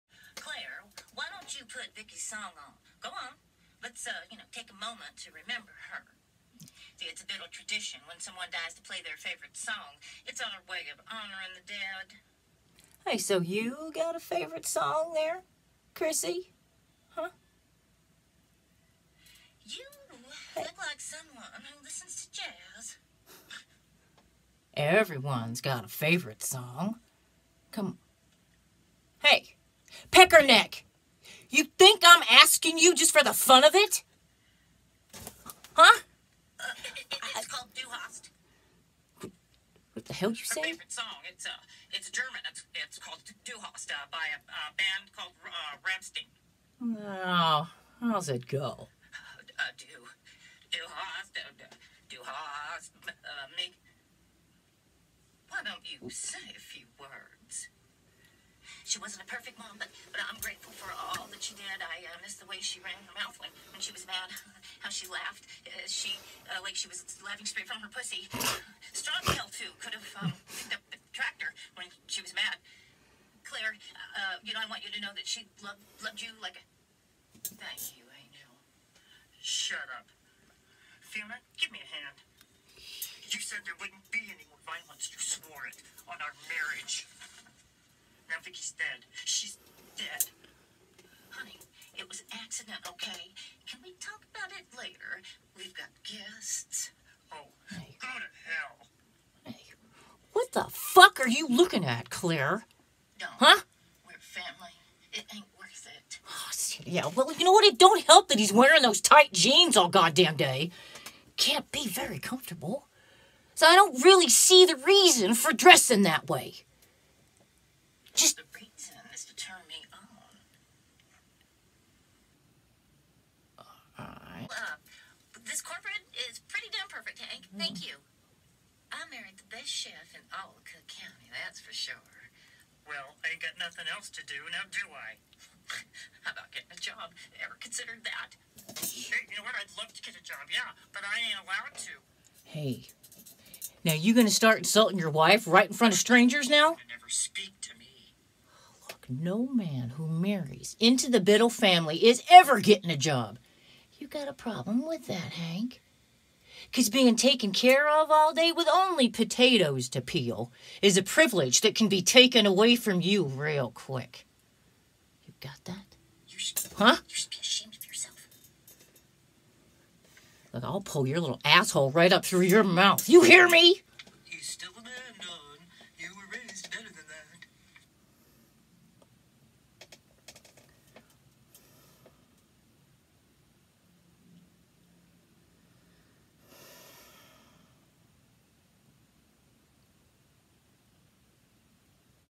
Audition: “Big, stupid, scary woman, Raw & Real”
“A big, stupid looking woman. Sadistic, aggressive & Scary” Afraid of her. Raw and real. Not feminine.